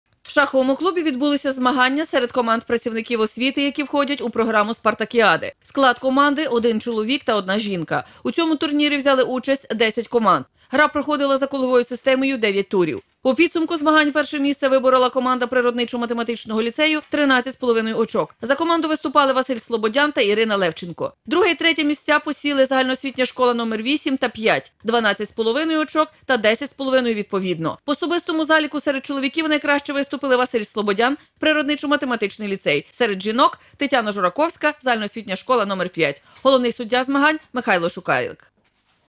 Хто із вчителів найкраще грає в шахи
Спартакіада вчителів міста стартувала в Коломиї. Учасниками спортивного дійства стали близько трьохсот педагогів. В рамках заходу змагалися і шахісти. Хто з вчителів найкраще грає в шахи - дивіться далі.